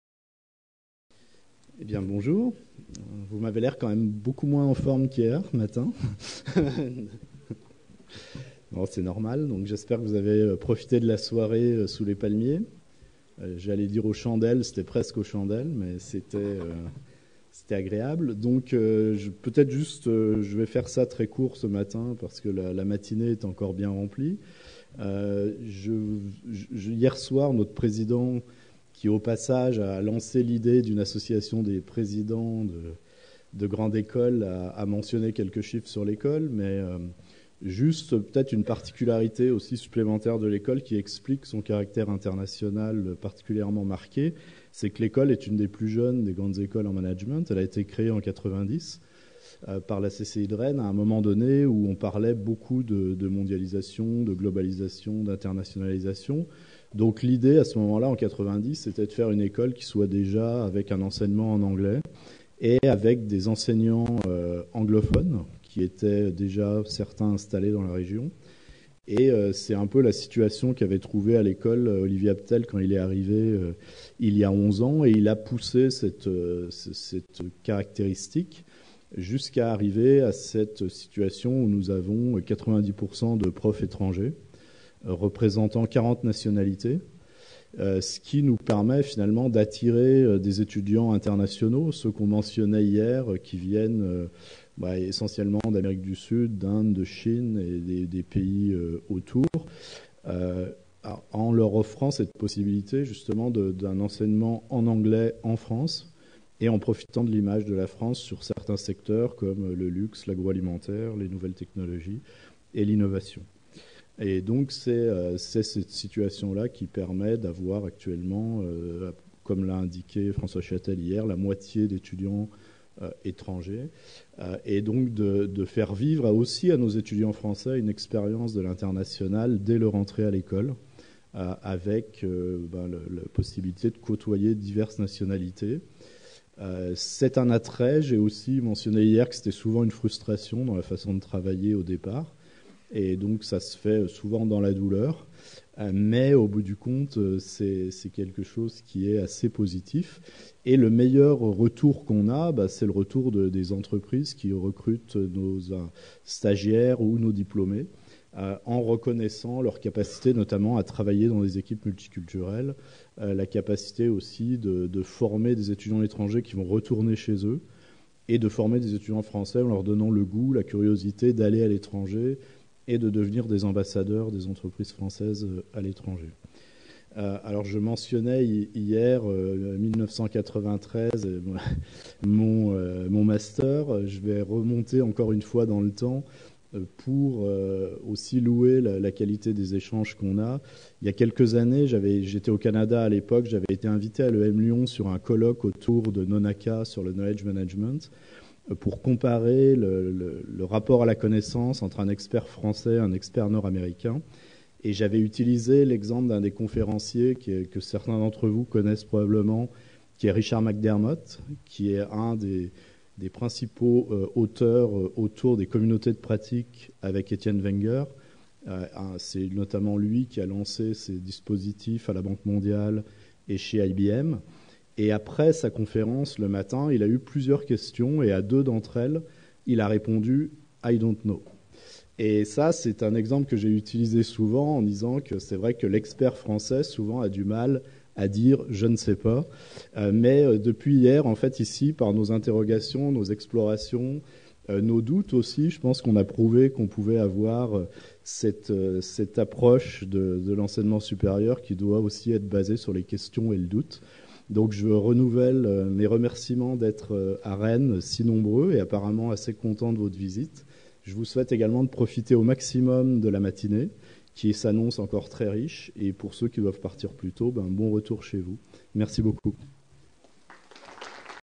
Congrès CGE - 06/10 /2017 - Accueil de J-M Viola
Allocution de Jean-Michel Viola, Acting Dean de Rennes School of Business lors du congrès CGE 2017.